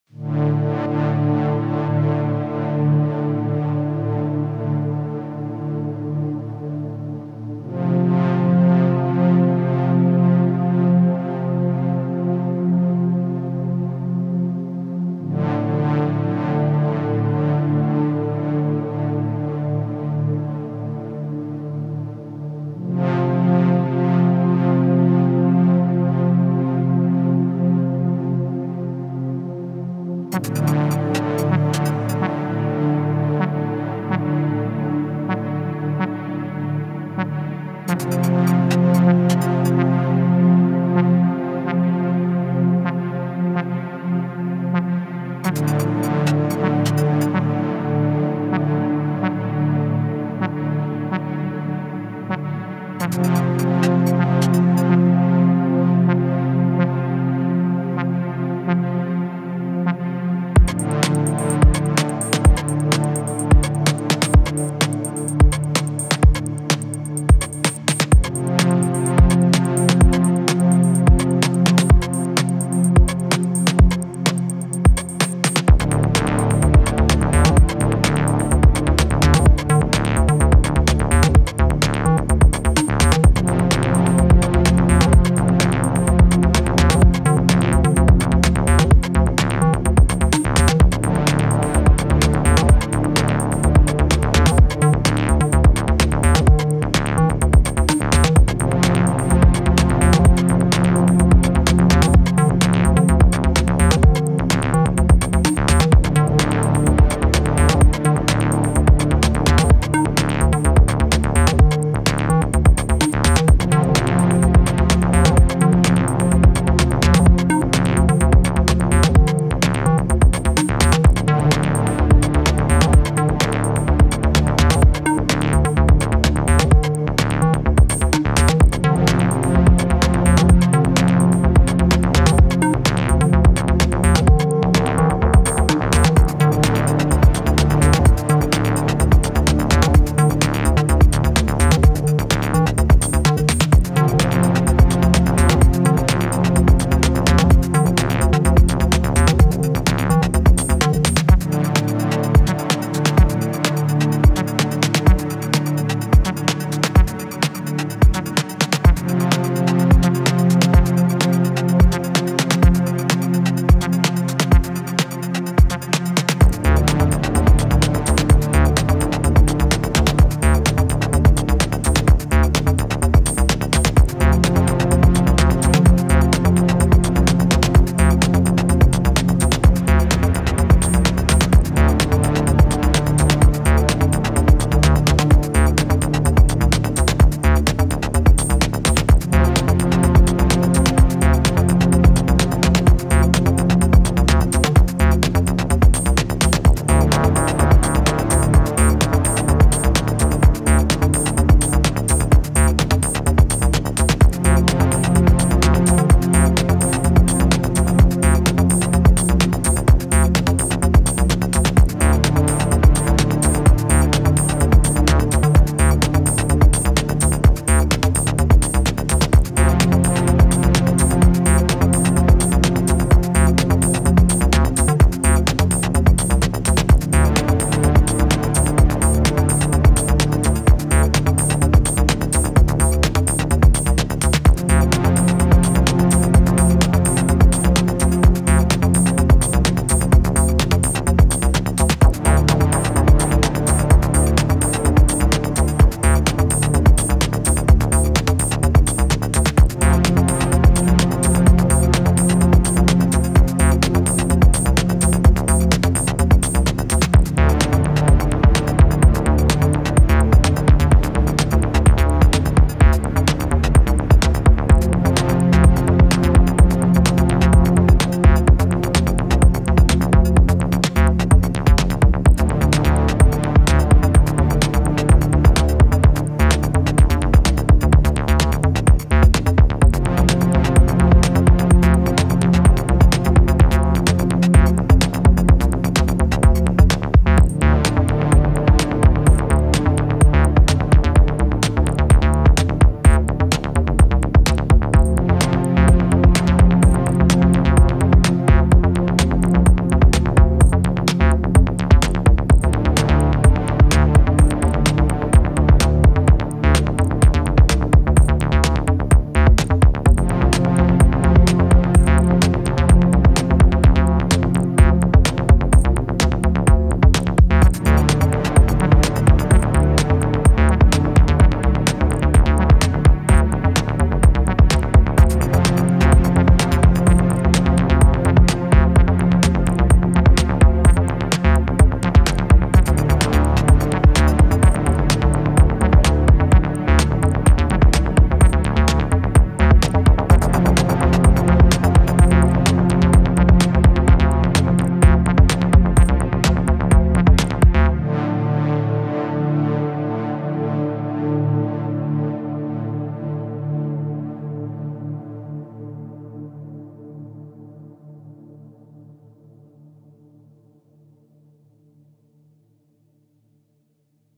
Thursdacid - a few Cirklon patterns for the Eurorack
I've been ill with a virus this weekend, so rather than plan a complex patch, I decided to just punch a few patterns into the Cirklon and make some acid breaks with the CV BOB sequencing the Eurorack.

This video pretty much has the patterns playing back. Everything in this video is sequenced by the Cirklon and the Euro is doing all the synthesis. The Cirklon is also manually sending via MIDI to a reverb and a delay in my DAW.